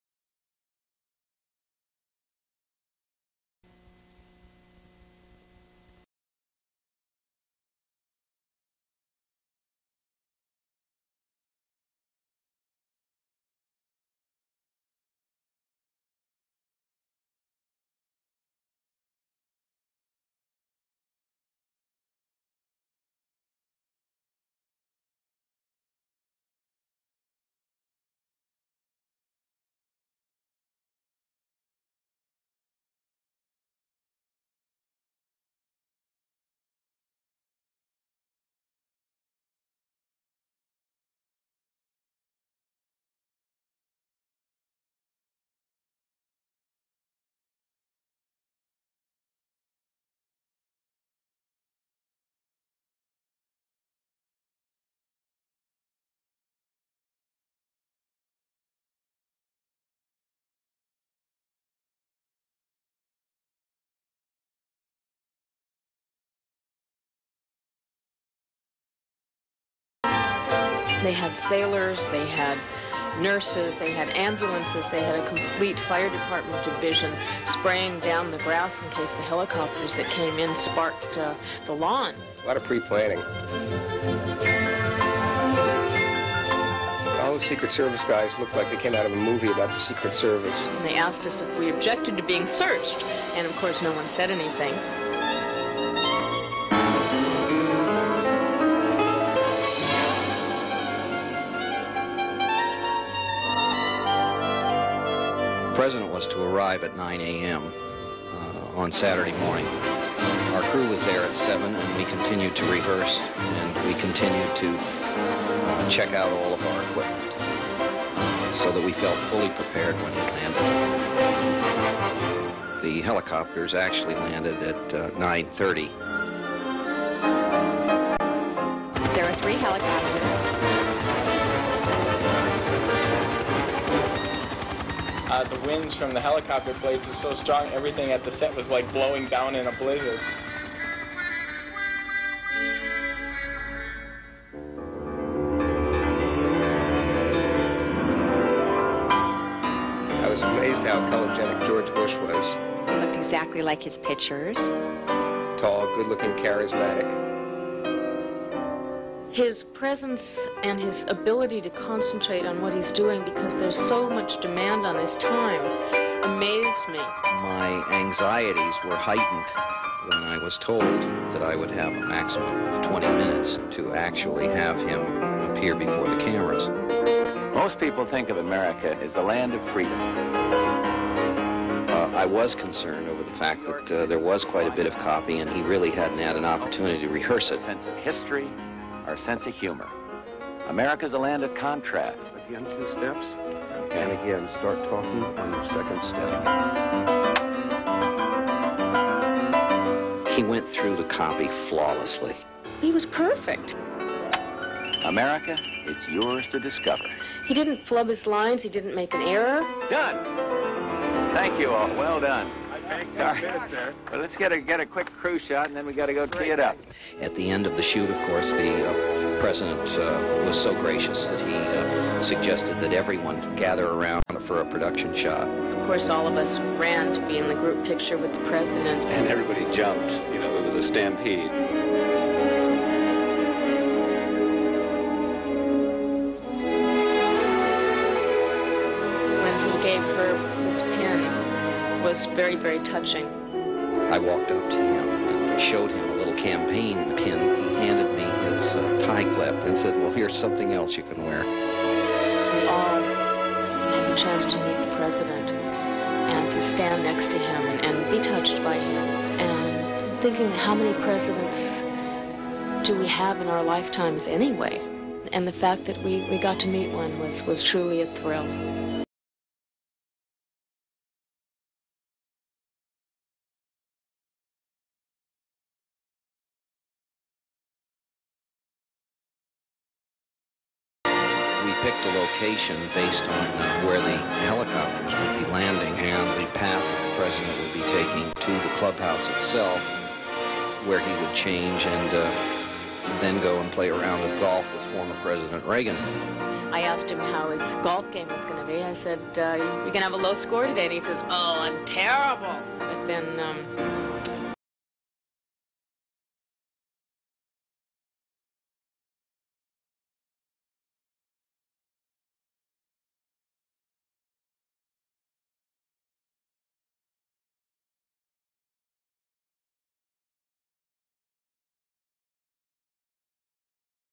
Herb Stott Films President George HW Bush.
Behind the scenes of a George Herbert Walker Bush spot.
Herbert Stott Director and owner of Stott Films Spungbuggy.